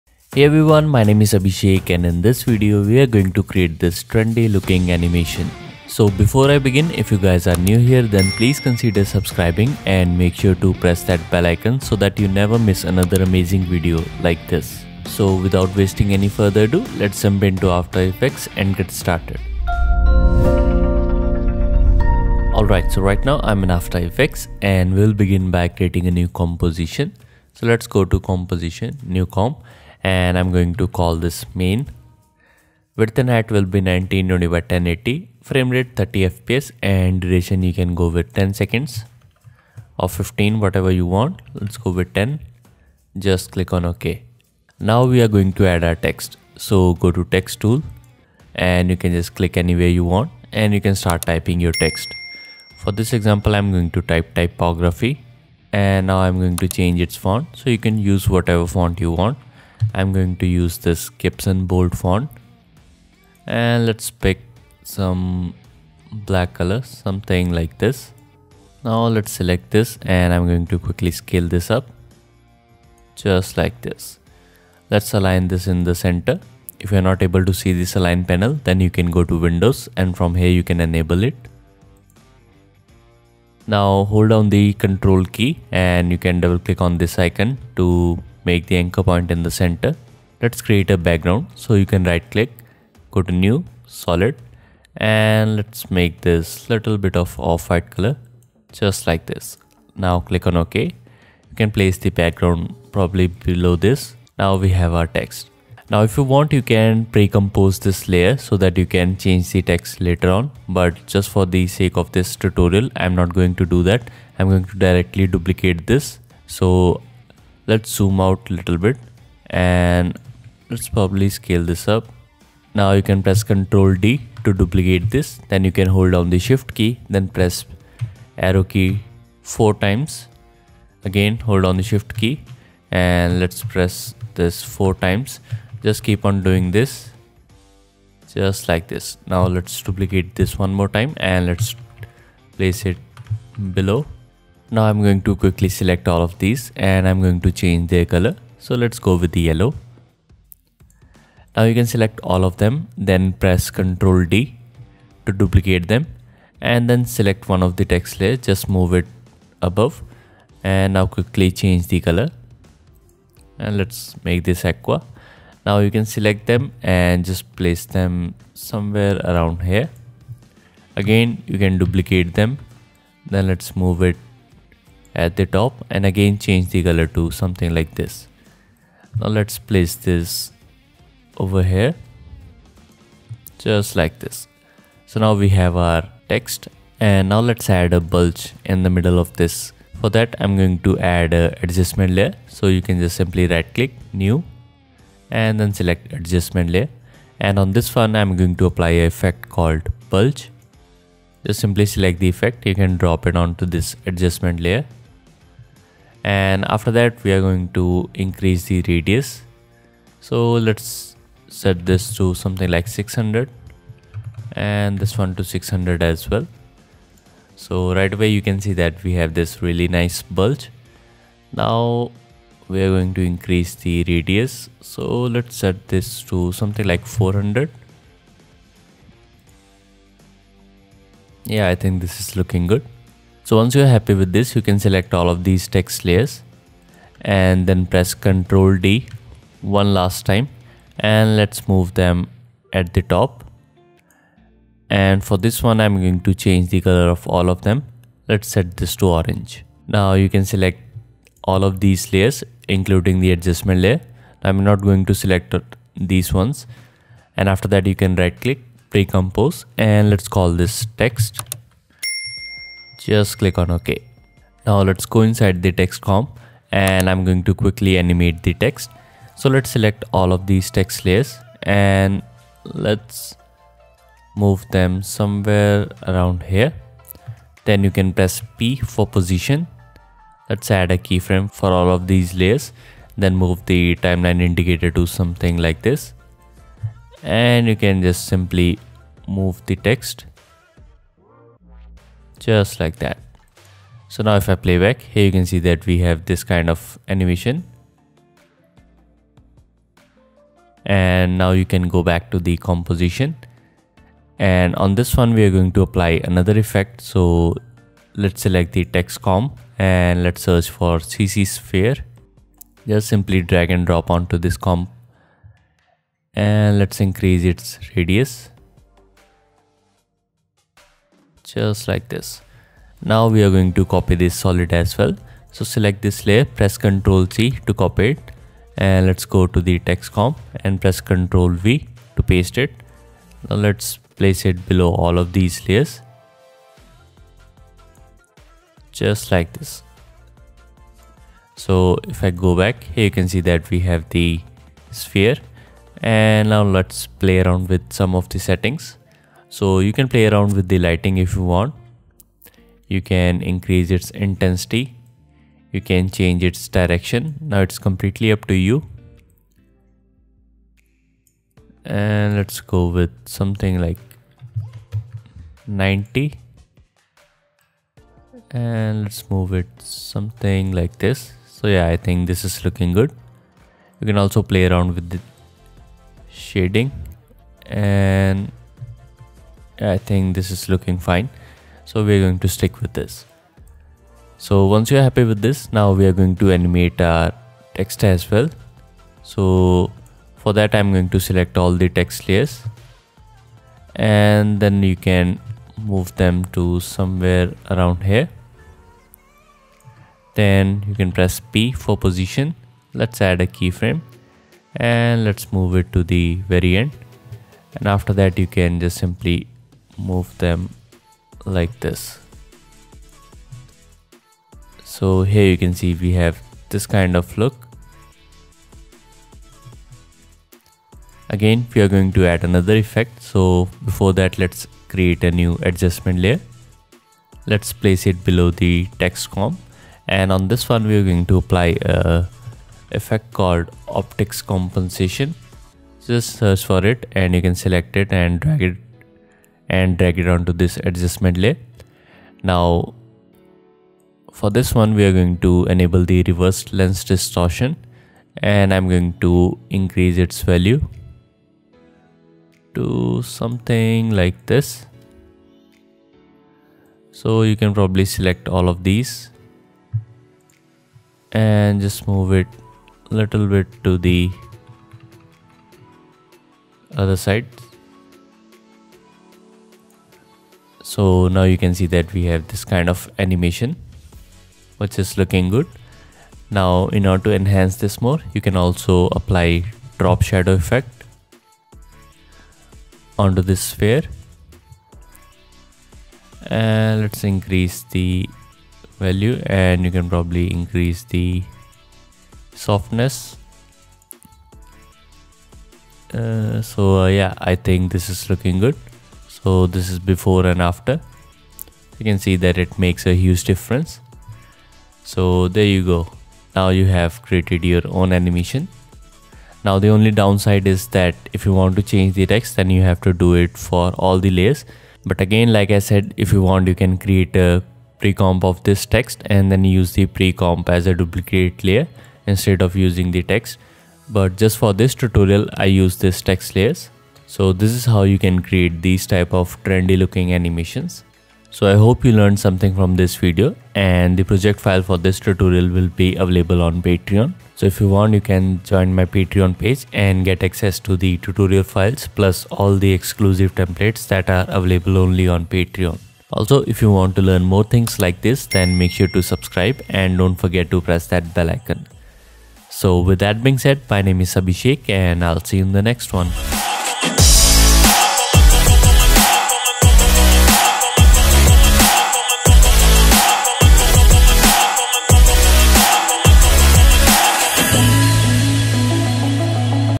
Trendy Typography Background Animation in After Effects | Creative Tuesdays - After Effects Tutorial
Create trendy typography animation in After Effects. Use this method to create an animated background and use it in your videos. Giving a modern and trendy look to your work.

Motion Nations